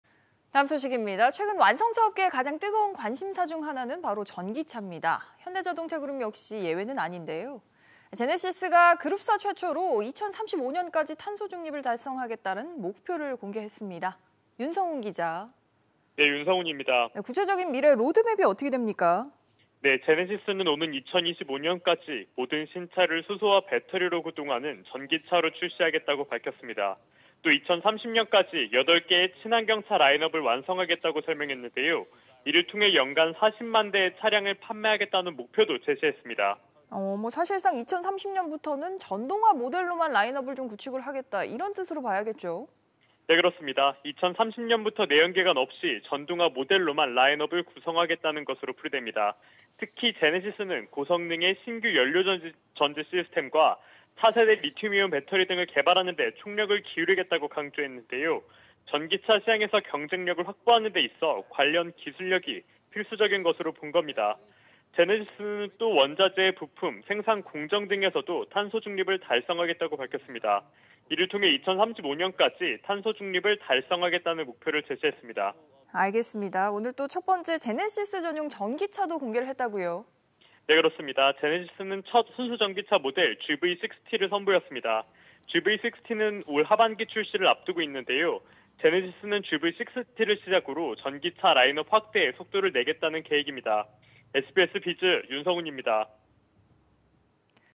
제네시스 “2025년부터 모든 신차는 수소·배터리 전기차”
[앵커]

완성차업계의 가장 뜨거운 관심사는 전기차입니다.

현대자동차그룹 역시 예외가 아닌데요.

제네시스가 그룹사 최초로 2035년까지 탄소 중립을 달성하겠다는 목표를 공개했습니다.

윤성훈 기자, 탄소중립 달성을 위한 제네시스의 구체적인 미래 로드맵이 어떻게 되는 겁니까?

[기자]

제네시스는 오는 2025년부터 모든 신차를 수소와 배터리로 구동하는 전기차로 출시하겠다고 밝혔습니다.